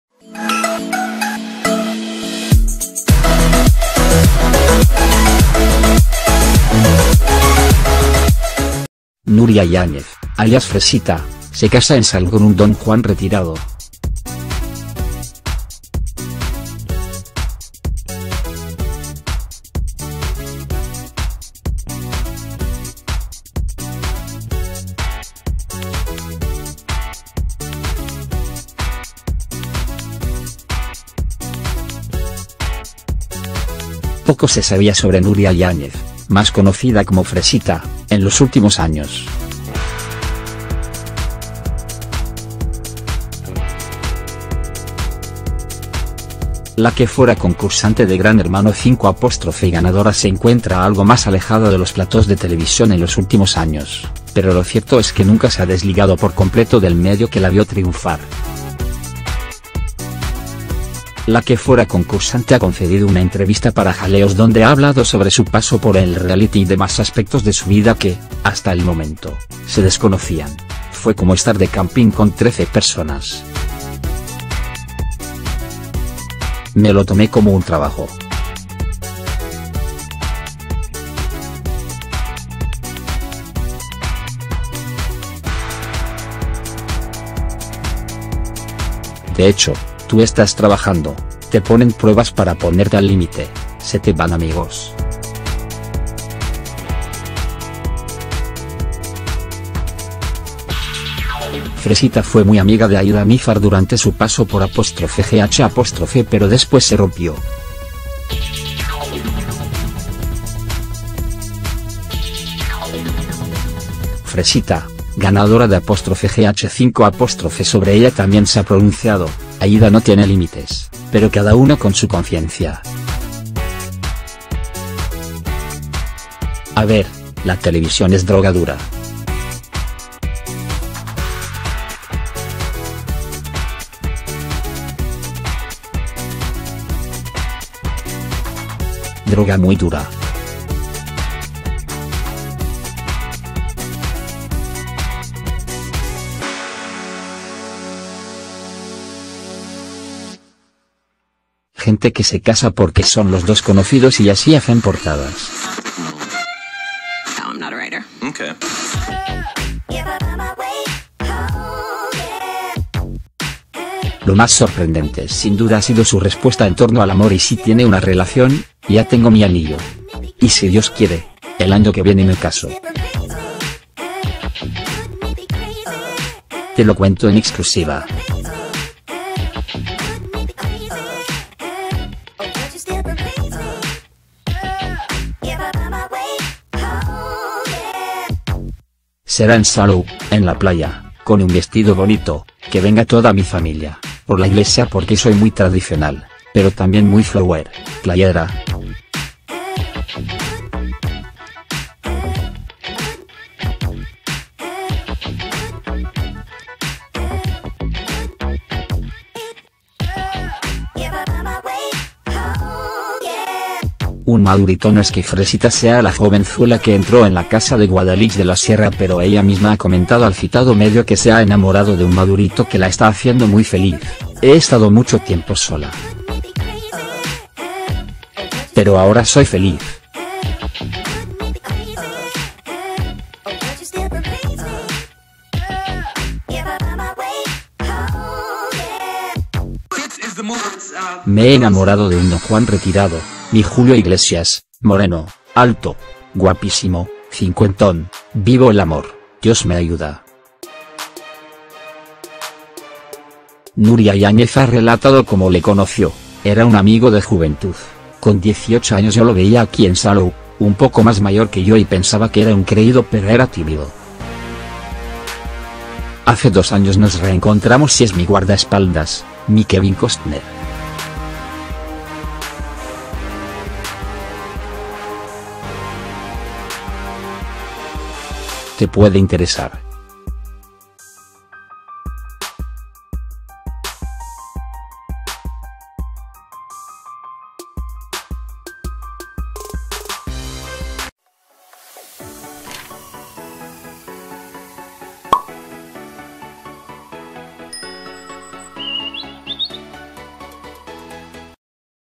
Nuria Yáñez, alias Fresita, se casa en Salou con un "donjuán retirado"
La que fuera concursante de 'GH' ha encontrado a su media naranja y lo ha confesado por sorpresa a un medio.